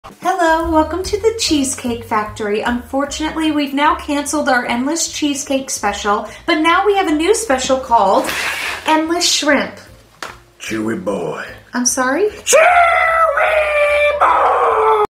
"The Janitor first word (My Version)"
The Janitor says he first word "CHEWY BOY!"